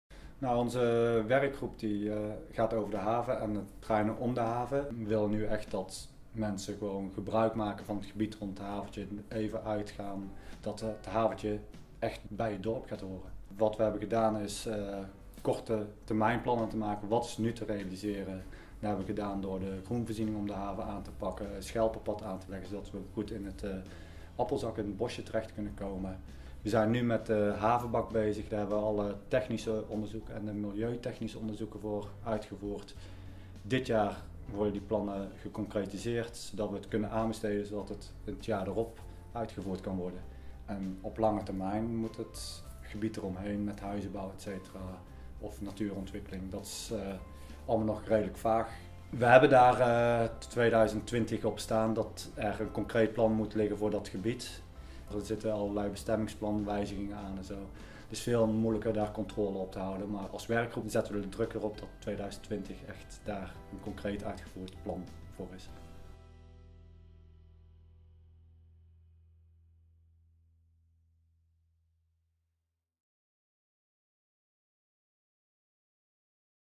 Ontwikkeling dorpshaven Moerdijk
In dit filmpje vertelt één van de inwoners van Moerdijk over de plannen voor de dorpshaven in Moerdijk. Bij de plannen voor  ontwikkeling van de haven is een werkgroep van de dorpstafel betrokken.